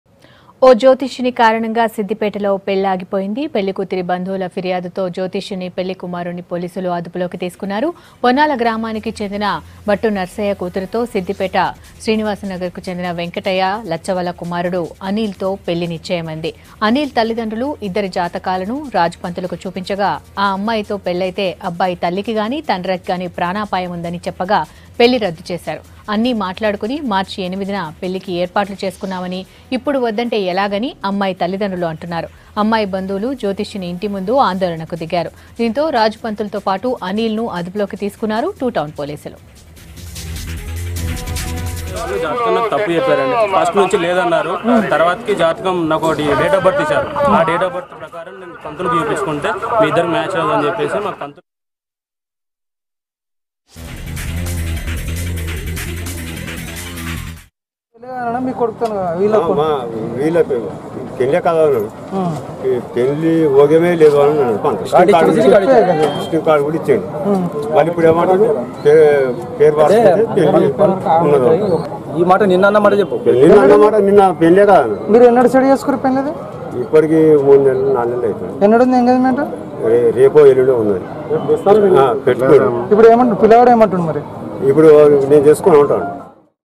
Bridegroom Canceled Wedding Due To Horoscope Issue In Siddipet | V6 News
Bridegroom Canceled Wedding Due To Horoscope Issue In Siddipet.